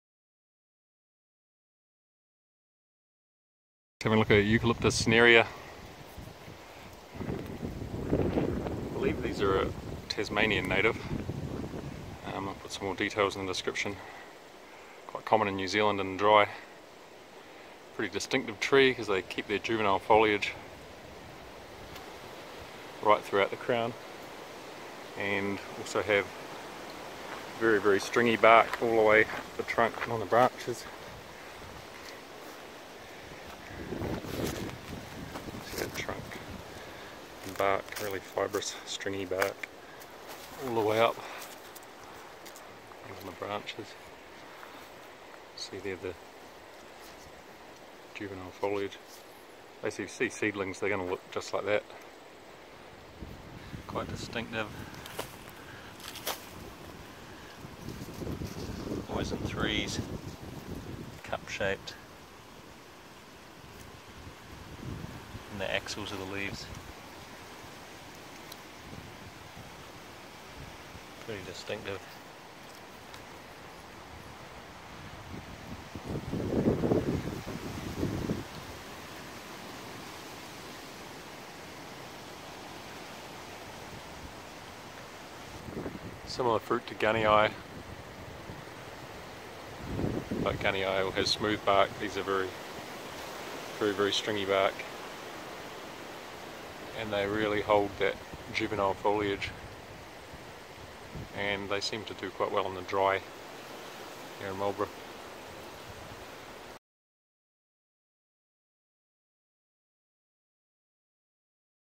Eucalyptus cinerea - Identification & Information
Eucalyptus cinerea, commonly known as the Argyle apple or mealy stringbark,[2] is a species of small to medium-sized tree that is endemic to south-eastern Australia. It has rough, fibrous bark on the trunk and branches, usually only juvenile, glaucous, egg-shaped leaves, flower buds in groups of three, white flowers and conical to bell-shaped fruit.

Eucalyptus cinerea is a tree that typically grows to a height of 15–30 m (49–98 ft) tall and forms a lignotuber. It has thick, fibrous, reddish brown to grey brown, longitudinally fissured bark on the trunk to the small branches. The leaves on young plants and on coppice regrowth are arranged in opposite pairs, sessile, glaucous, broadly egg-shaped to more or less round, up to 80 mm (3.1 in) long and 50 mm (2.0 in) wide. Intermediate leaves are arranged in opposite pairs, glaucous, egg-shaped to lance-shaped, 48–90 mm (1.9–3.5 in) long and 20–45 mm (0.79–1.77 in) wide on a petiole 4–15 mm (0.16–0.59 in) long. Adult leaves are arranged alternately, lance-shaped, 90–140 mm (3.5–5.5 in) long and 15–50 mm (0.59–1.97 in) wide on a petiole up to 11 mm (0.43 in) long. The flower buds are arranged in groups of three in leaf axils on a peduncle 2–9 mm (0.079–0.354 in) long, the individual buds sessile or on a pedicel up to 3 mm (0.12 in) long. Mature buds are glaucous, diamond-shaped, 6–8 mm (0.24–0.31 in) long and 3–5 mm (0.12–0.20 in) wide with a conical operculum. Flowering occurs between May and November and the flowers are white. The fruit is a woody, conical to bell-shaped capsule 4–7 mm (0.16–0.28 in) long and 5–9 mm (0.20–0.35 in) wide with the valves level with the rim or slightly beyond.